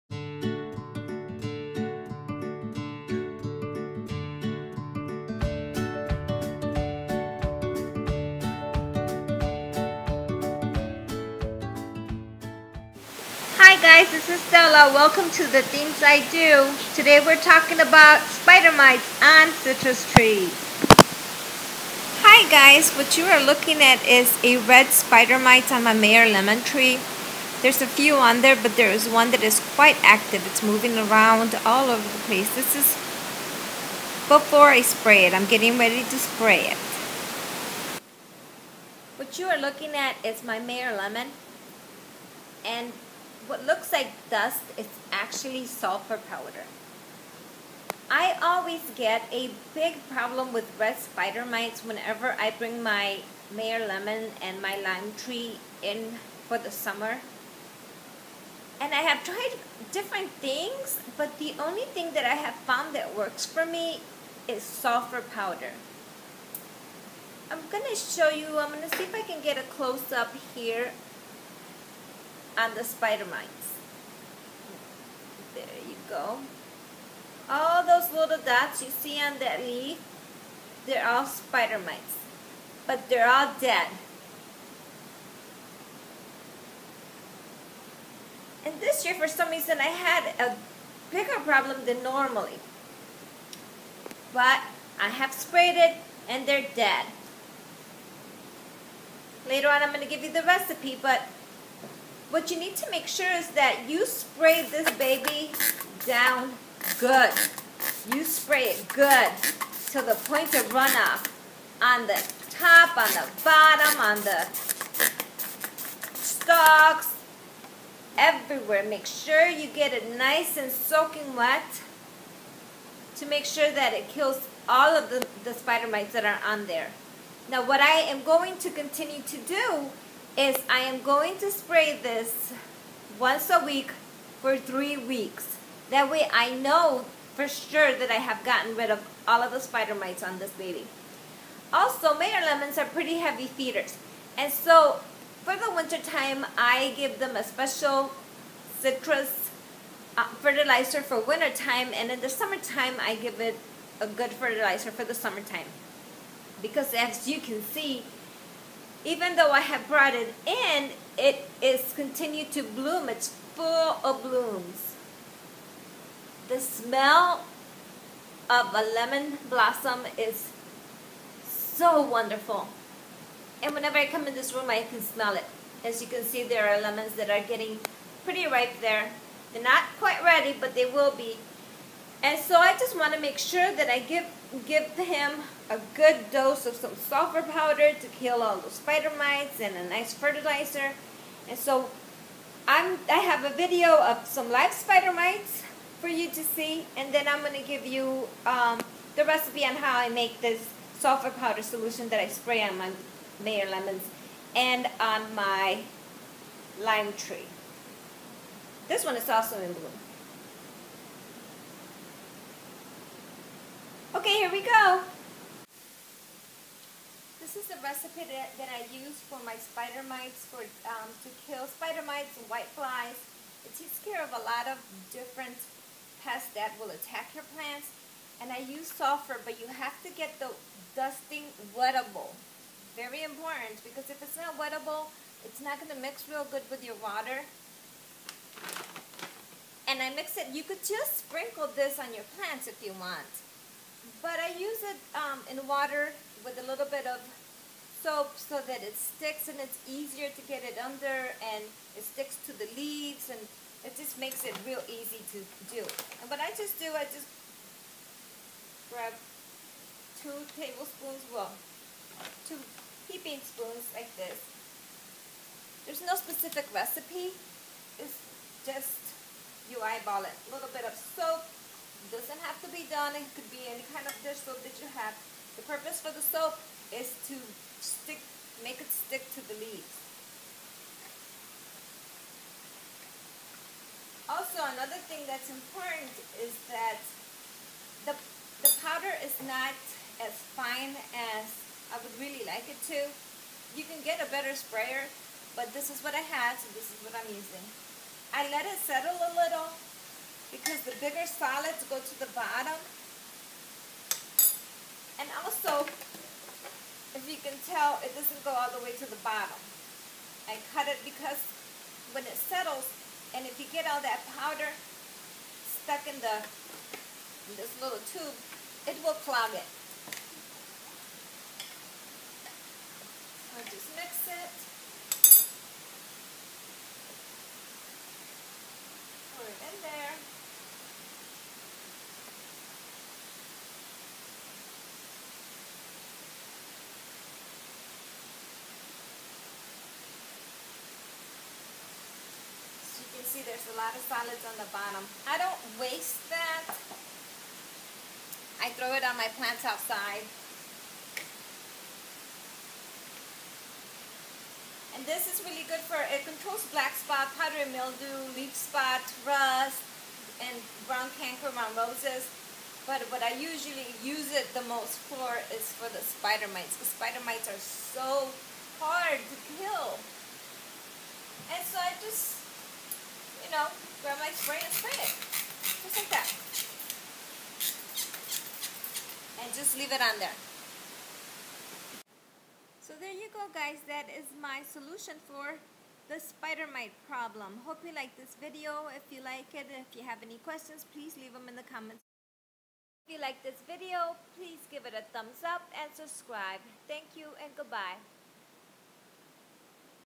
How I treat Meyer lemon and citrus trees for spider mites.
How to get rid of red spider mites on citrus trees.
I use powder sulfur with a little bit of soap on a spray bottle.
Meyer lemon.